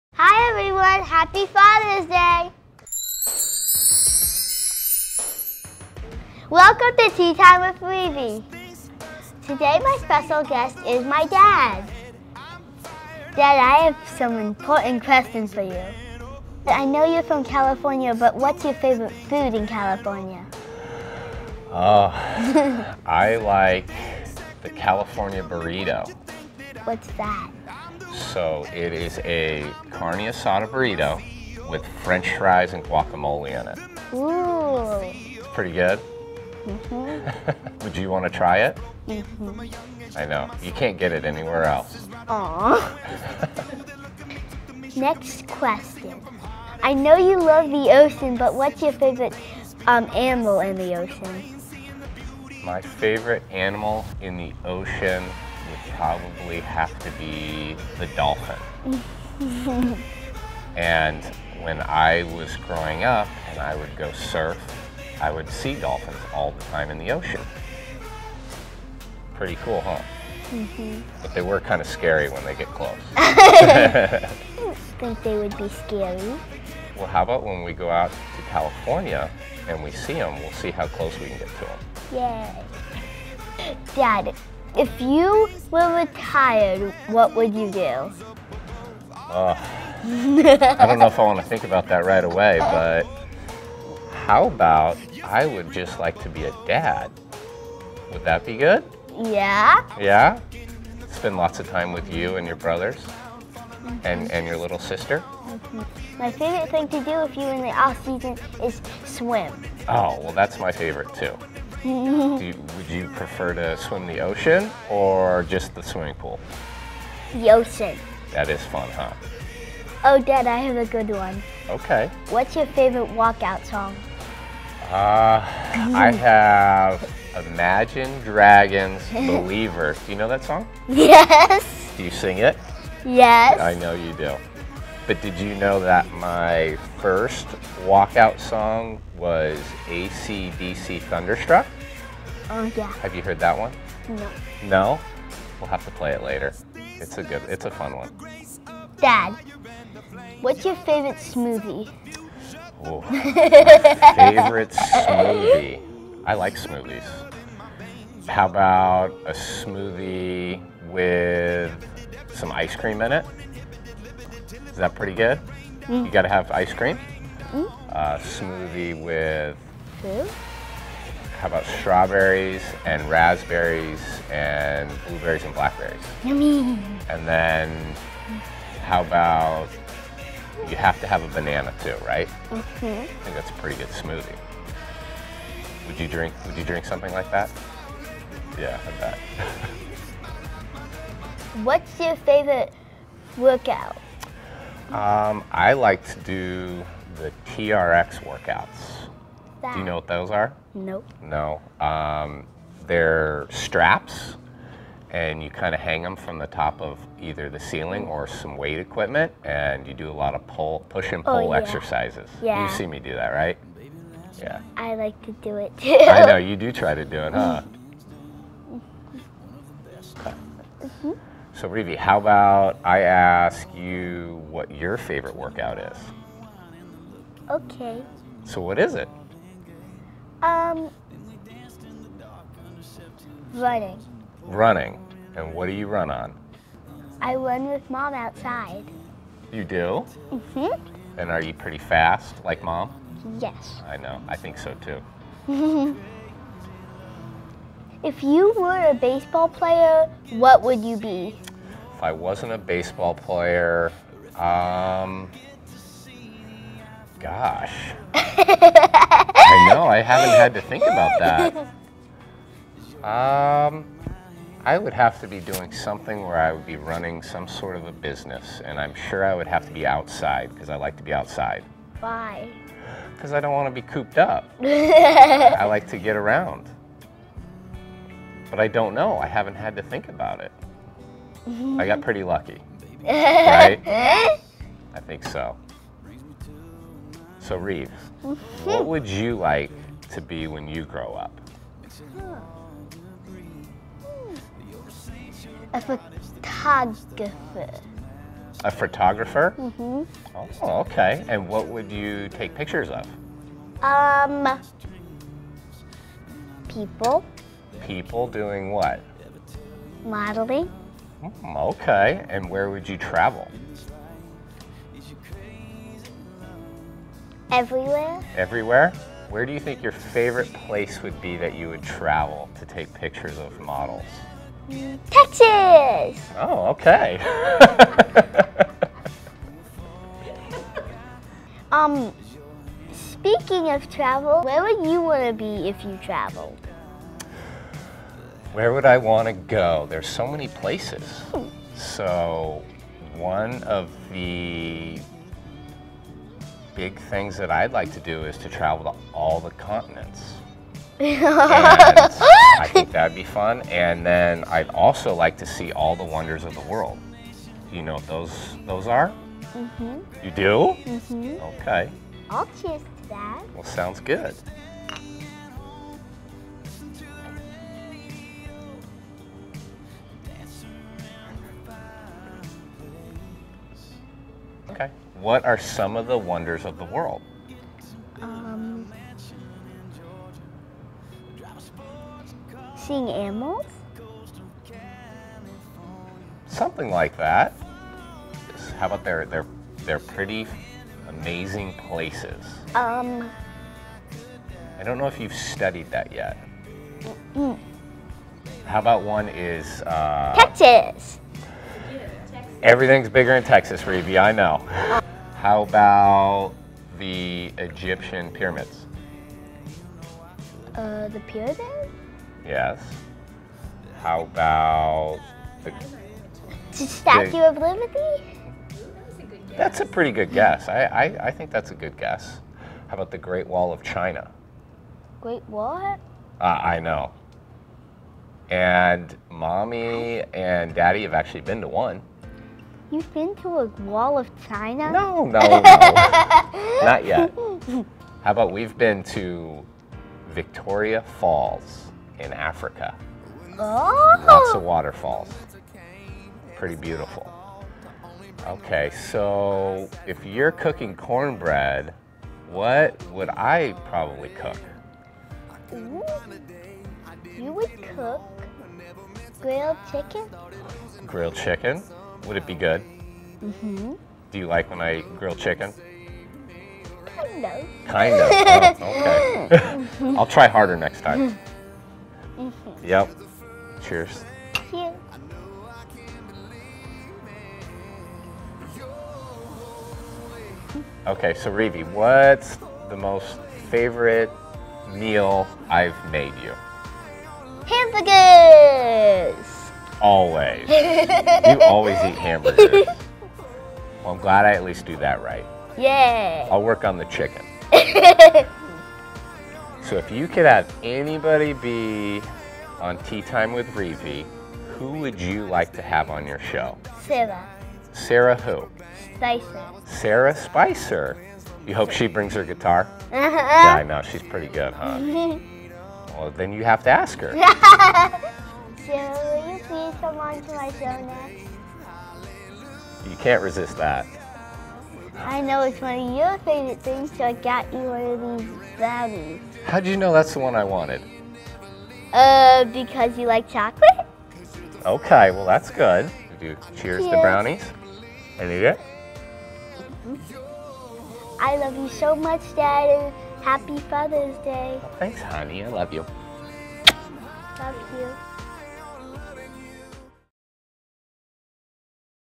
Tea Time with Reevey: Father's Day Edition with Cole Hamels!
Laugh along to my interview with my dad, Texas Rangers' pitcher: Cole Hamels!

We talk about life, love of burritos and have some LOL moments :)